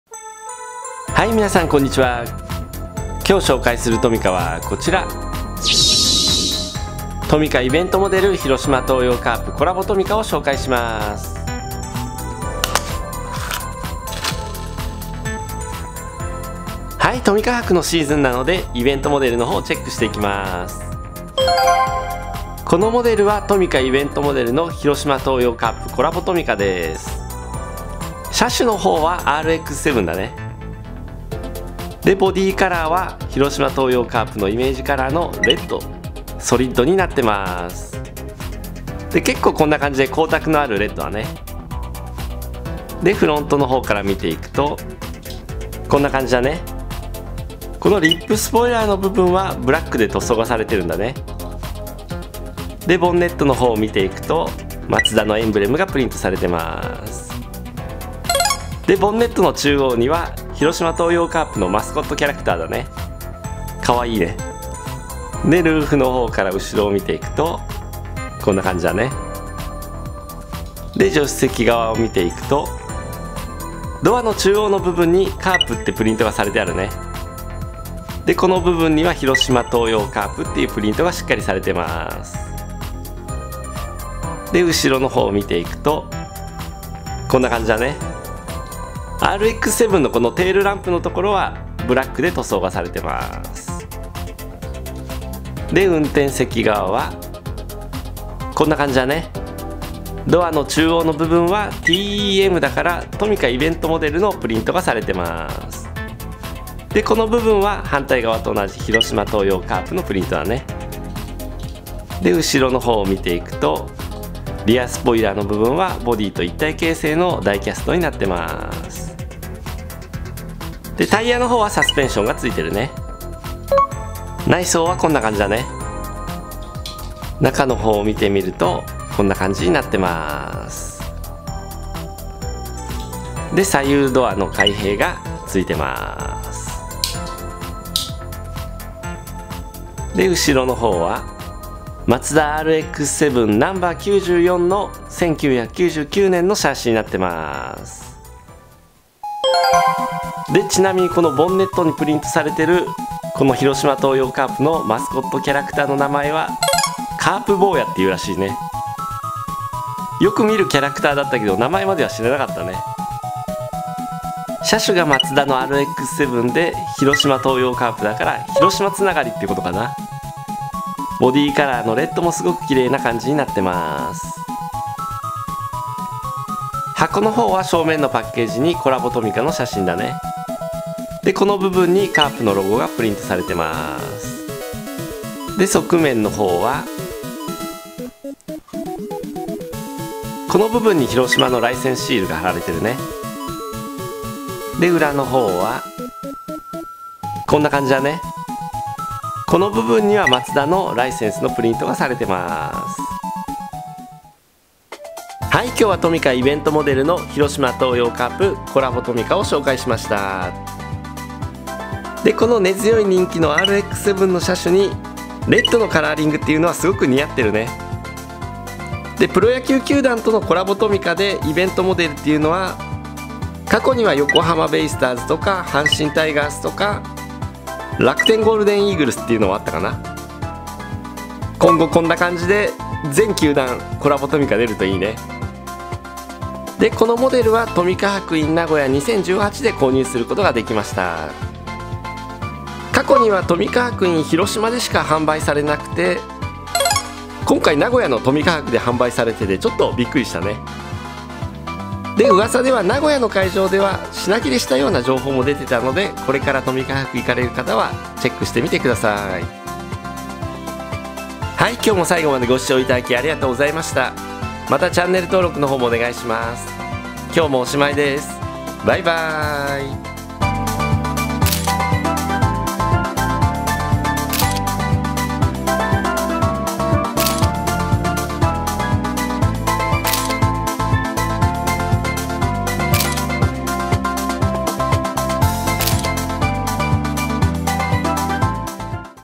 トミカ博in名古屋で品切れ！？【イベントモデル】【コラボトミカ】広島東洋カープ×トミカ
まさか名古屋会場でも販売されるとは･･･サプライズ！！

★関連動画
【速報】トミカ博 in YOKOHAMA 品切れ情報　ラ・フェラーリ 買えませんでした･･･　トミカイベントモデル No 27

          効果音ラボ